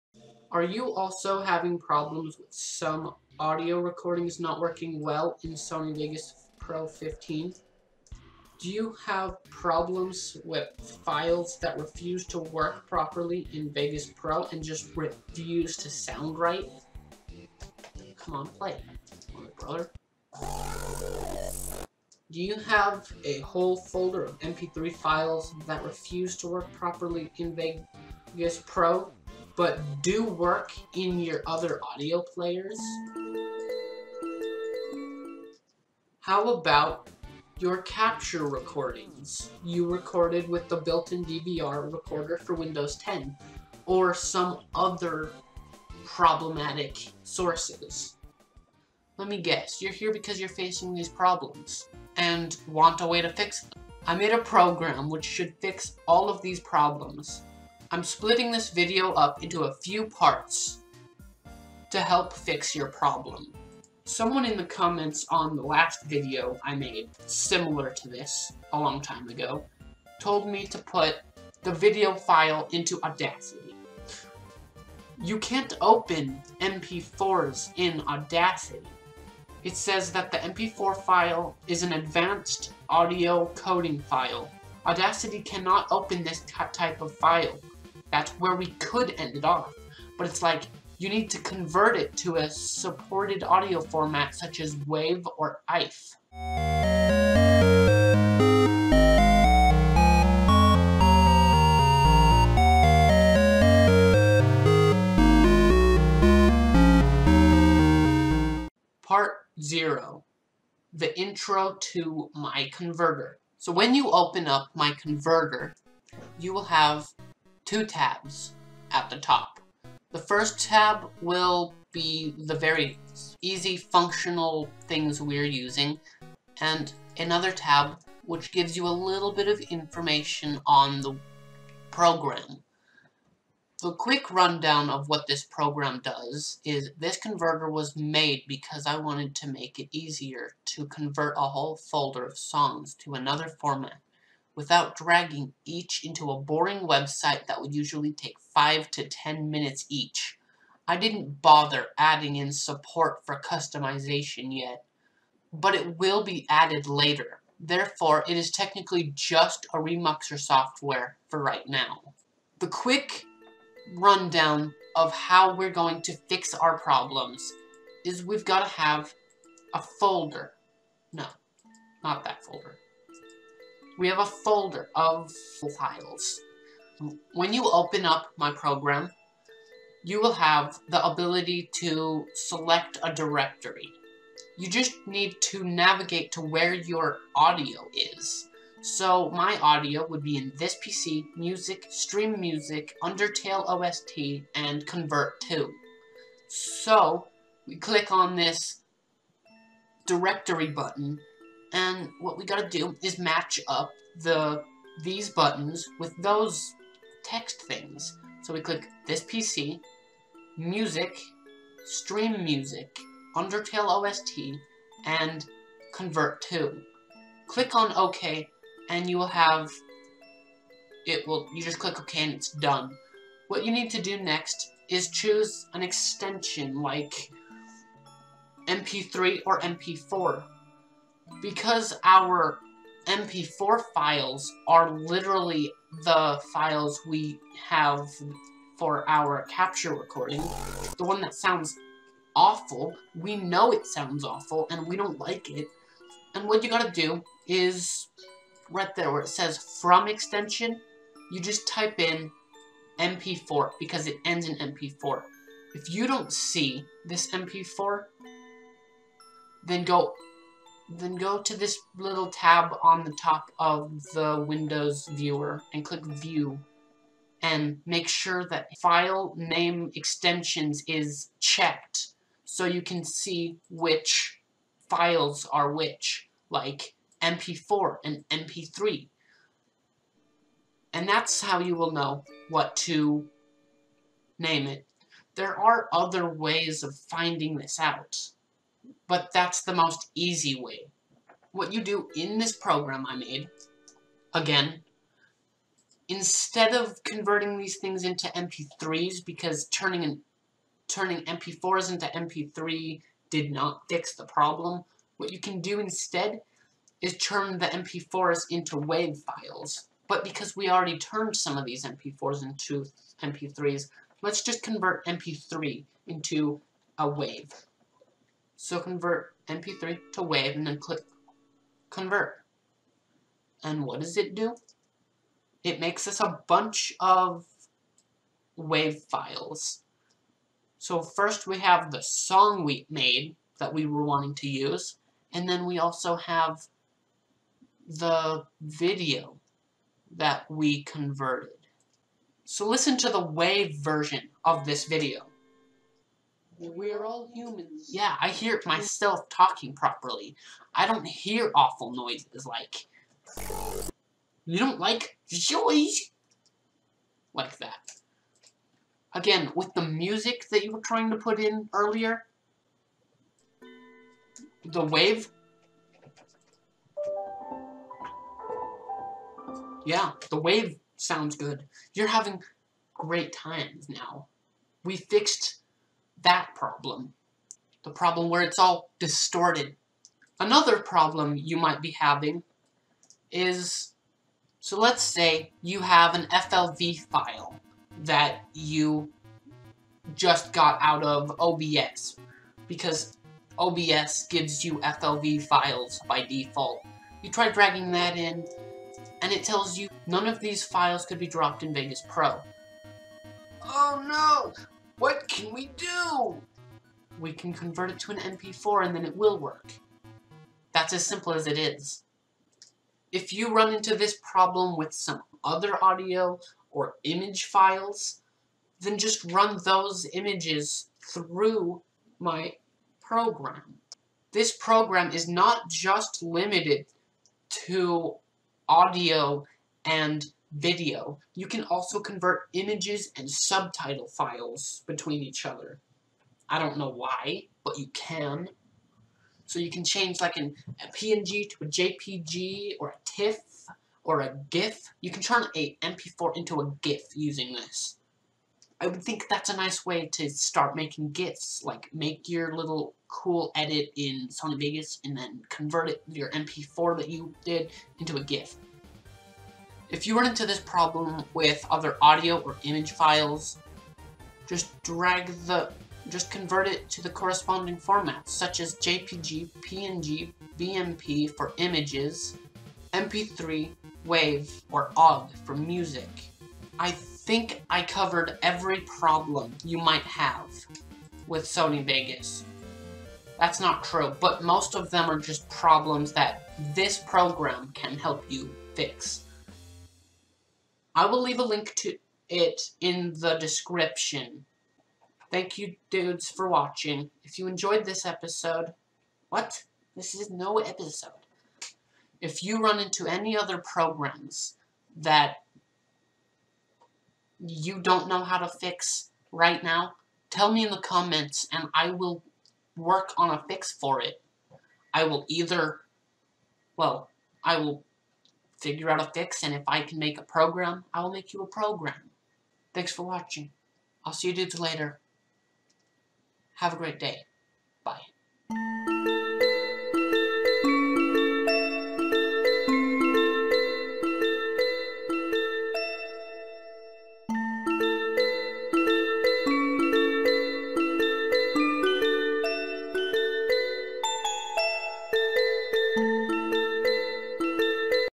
How to fix most vegas pro 15 audio problems 2018 + my remux software
I made a converter/remux software that will make it easier to fix vegas pro file problems, but you can also use it to convert a whole folder of Pictures, video or audio to another format.

The link to the software is going to be up by the time the video is out here (Sorry about the ads, goo.gl is becoming discontinued):